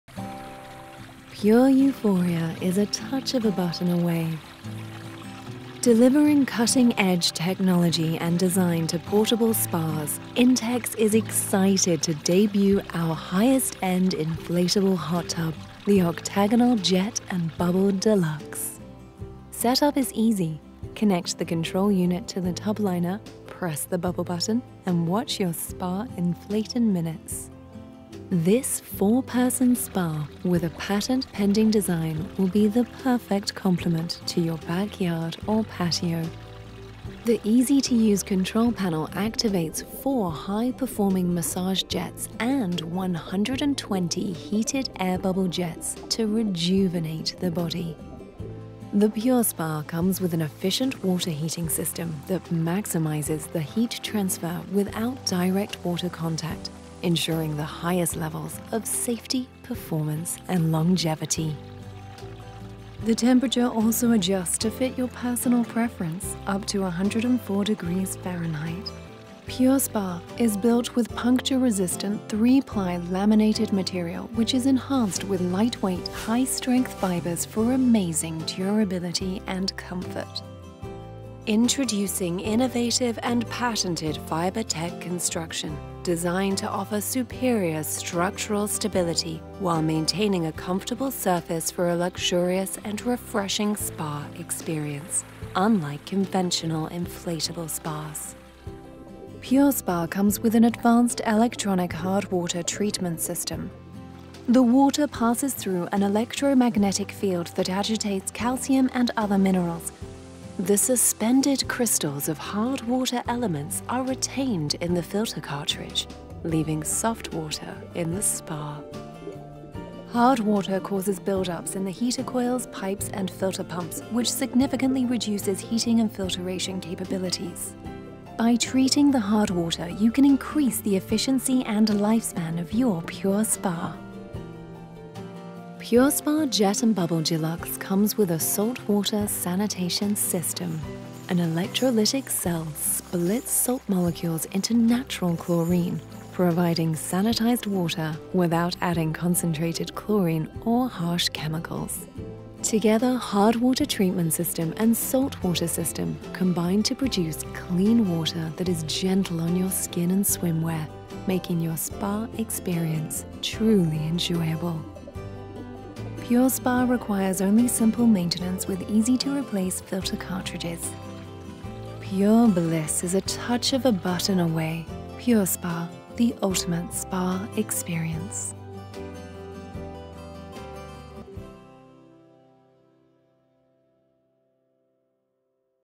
Intex® PureSpa™ Jet & Bubble Deluxe
The PureSpa™ Jet and Bubble Deluxe Hot Tub combines the pleasures of a high-performance jet massager and a bubble massage spa in one luxurious setting. High-performance engineering and Hydro Aeration® Technology make the PureSpa™ Jet and Bubble Deluxe Hot Tub the gold standard in luxury spas. Intex's Saltwater Sanitation and Hard Water Treatment systems come standard and ensure an effortless spa experience.